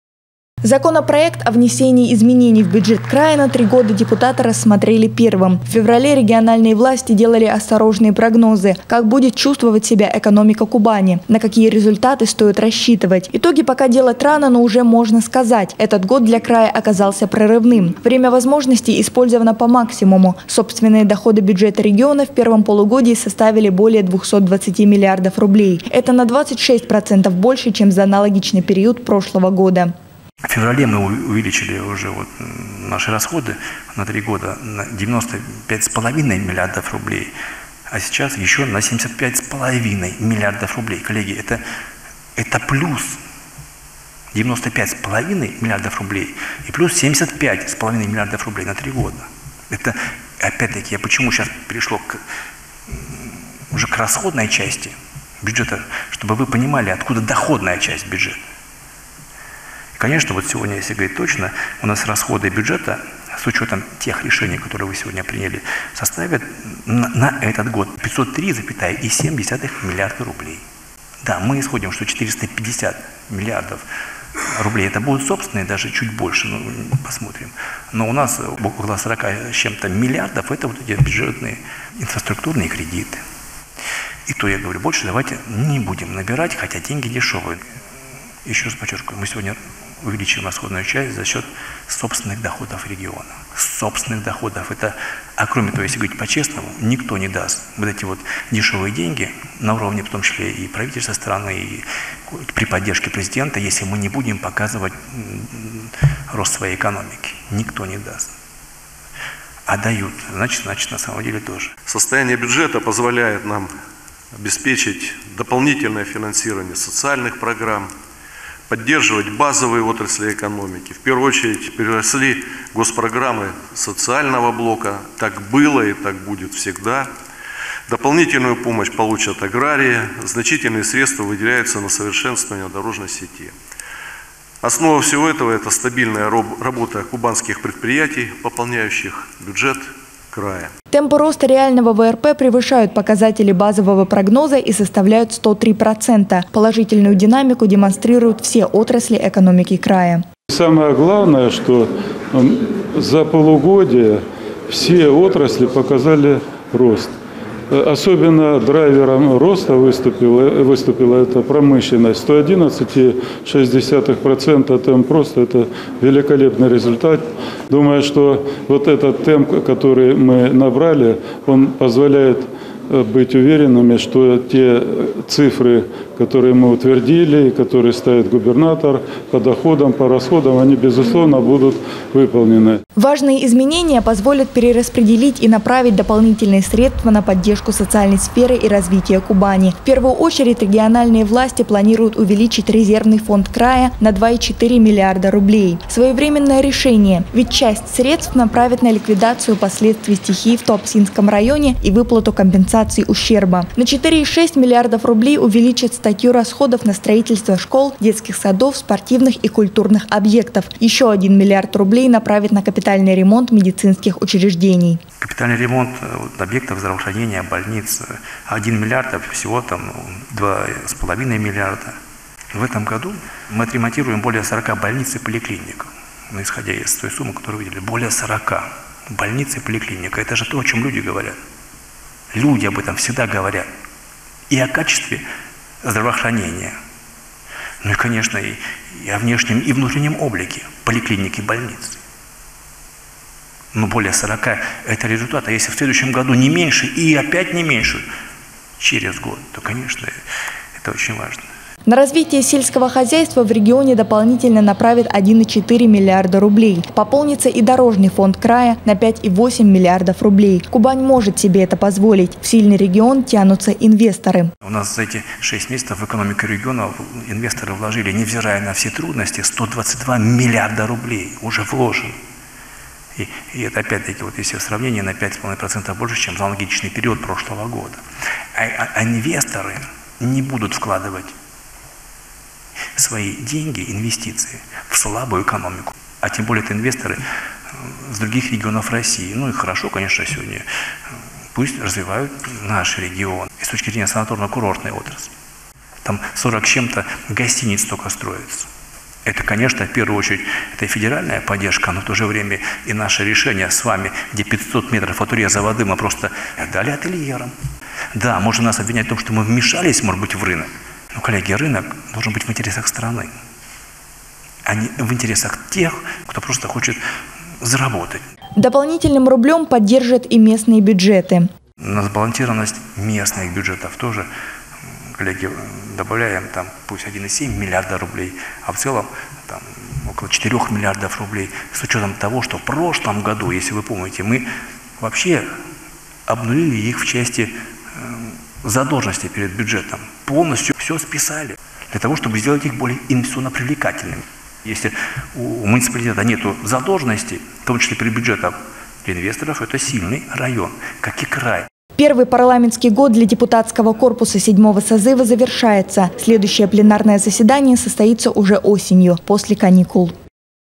Сессия ЗСК
Расходы краевой казны на три года увеличили на более чем 75 млрд рублей. Изменения в бюджет региона приняли на сессии Законодательного собрания. В заседании принял участие губернатор Краснодарского края. Вениамин Кондратьев отметил, что с учетом принятых поправок расходы краевого бюджета в этом году составят почти 503 миллиарда рублей.
Законопроект о внесении изменений в бюджет края на 3 года депутаты рассмотрели первым.. Собственные доходы бюджета региона в первом полугодии составили более 220 млрд рублей. Это на 26% больше, чем за аналогичный период прошлого года.
Темпы роста реального ВРП превышают показатели базового прогноза и составляют 103%. Положительную динамику демонстрируют все отрасли экономики края.
Важные изменения позволят перераспределить и направить дополнительные средства на поддержку социальной сферы и развития Кубани. В первую очередь, региональные власти планируют  увеличить резервный фонд края - на 2,4 млрд рублей. Своевременное решение.  Ведь часть средств направят на ликвидацию последствий стихии в Туапсинском районе и выплату компенсаций ущерба. На 4,6 млрд рублей увеличат статью расходов на строительство школ, детских садов, спортивных и культурных обьектов. Еще 1 млрд рублей направят на капитальный ремонт медицинских учреждений.
Первый парламентский год для депутатского корпуса 7 созыва завершается. Следующее пленарное заседание состоится уже осенью, после каникул.